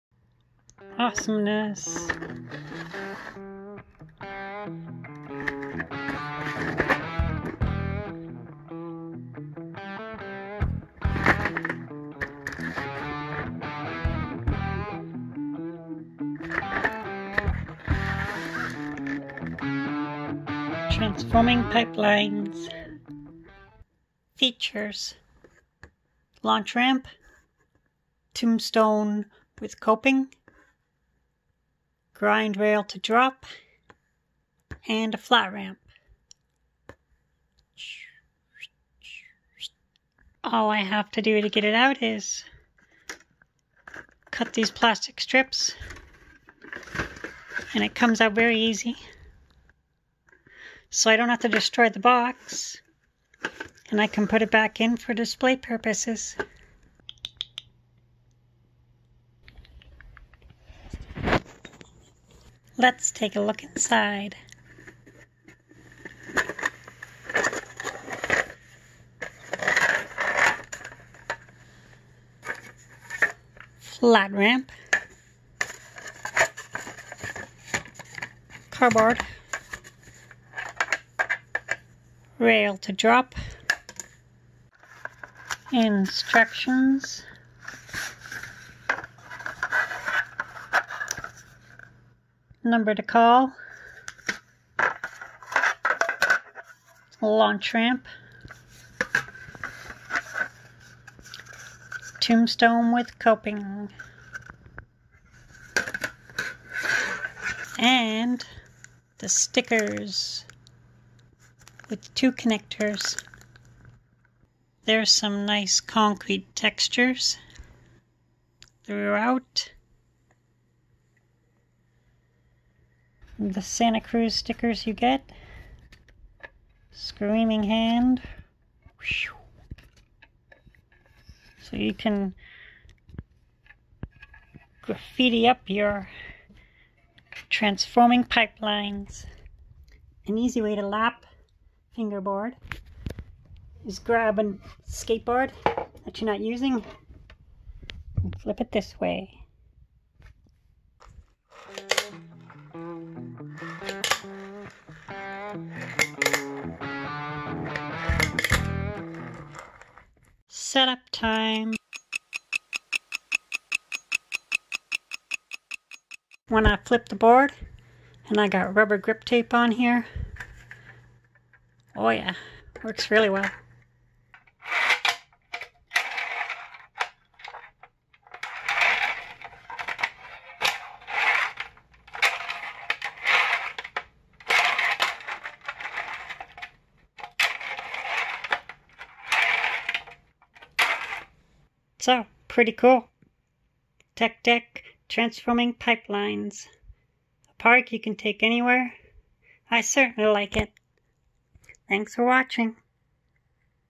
Tech Deck Transforming Pipelines
I'm back, skateboarding clips coming soon too. 🛹🙌😊 A look at Tech Deck Transforming Pipelines park. And how to fingerboard on your lap in comfort, couch style.